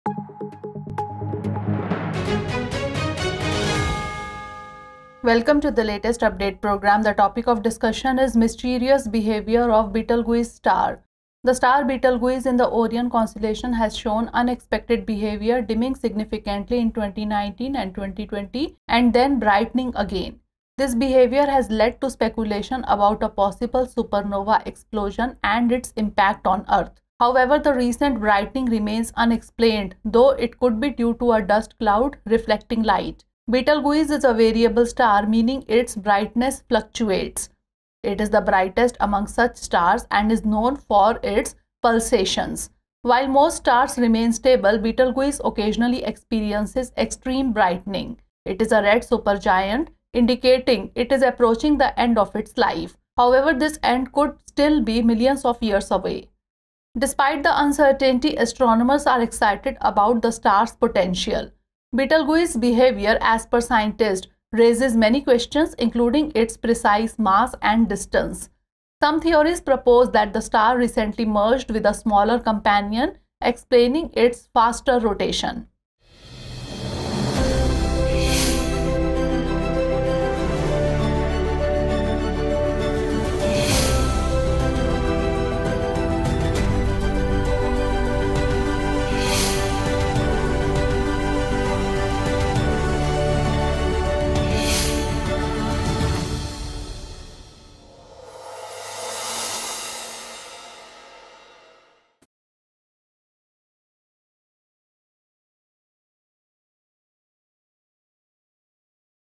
Mysterious Behavior Of Betelgeuse Star: Latest update | Drishti IAS English
The star Betelgeuse in the Orion constellation has shown unexpected behavior, dimming significantly in 2019 and 2020 and then brightening again, which has led to speculation about a possible supernova explosion and its impact on Earth.